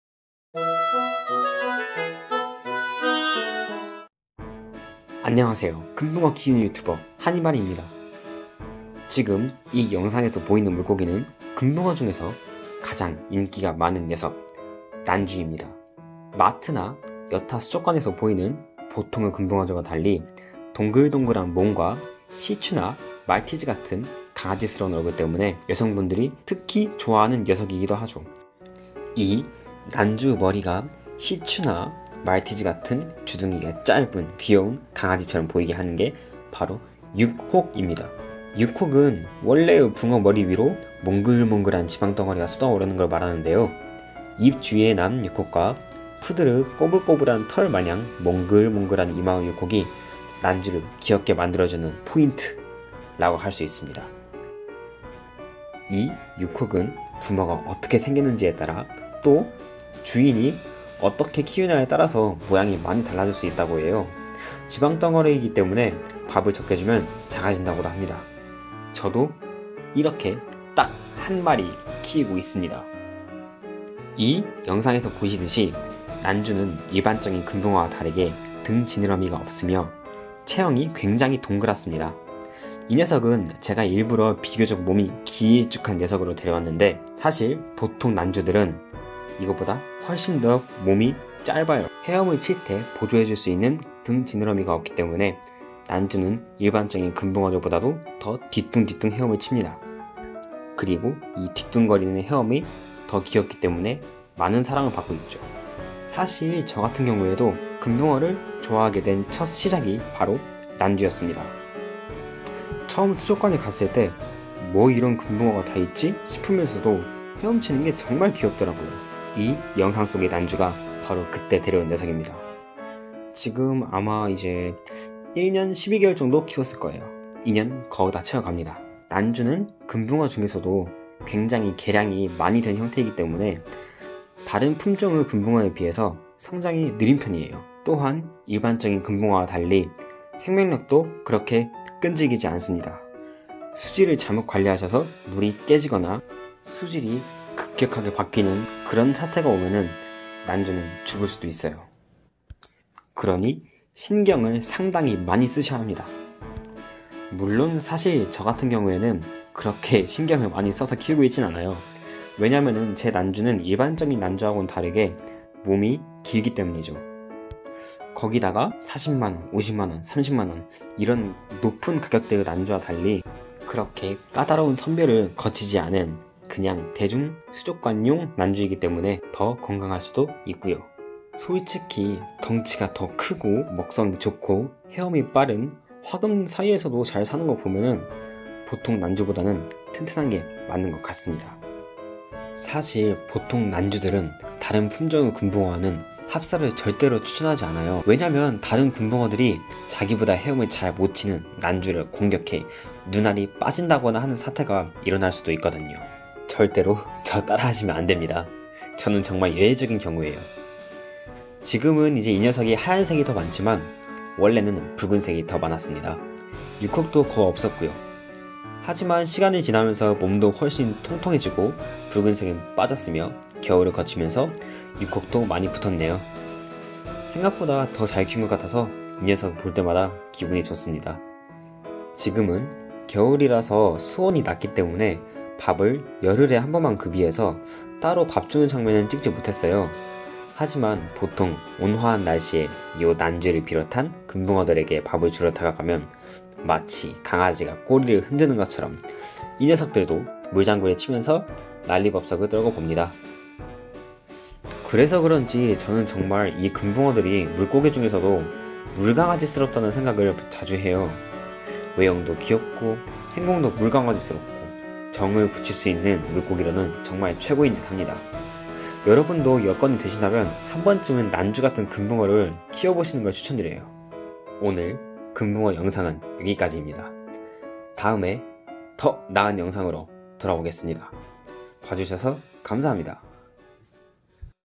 무조건 키워야 하는 강아지 같은 금붕어!! 아직도 안키우세요?
관상어 박람회로 인해 금붕어의 인지도가 점점 더 높아져가고 있고
그 중에서도 가장 인기가 많은 품종이 난주더라구요
저도 난주에 반해 금붕어를 시작했으니..정말 매력적인 관상어인것 같습니다^^